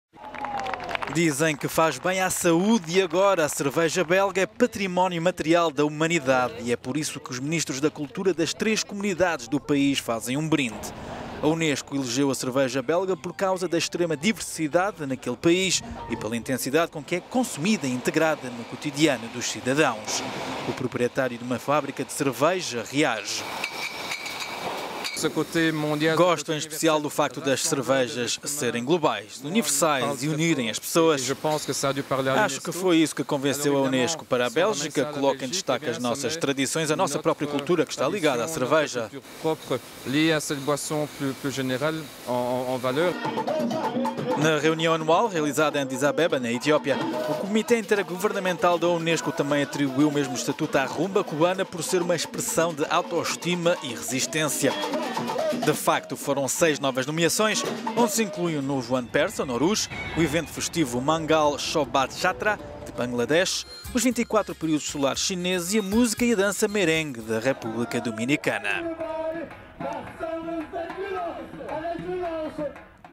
Cerveja belga é património imaterial da humanidade
Dizem que faz bem à saúde e agora a cerveja belga é Património Imaterial da Humanidade.

A UNESCO elegeu a cerveja belga por causa da "extrema diversidade naquele país e pela intensidade com que é consumida e integrada no quotidiano dos cidadãos".

O proprietário de uma fábrica de cerveja reagiu. "Gosto em especial do facto das cervejas serem globais, universais, e unirem pessoas. Acho que foi isso convenceu a UNESCO. Para a Bélgica é bom porque coloca em destaque as nossas tradições, a nossa pr…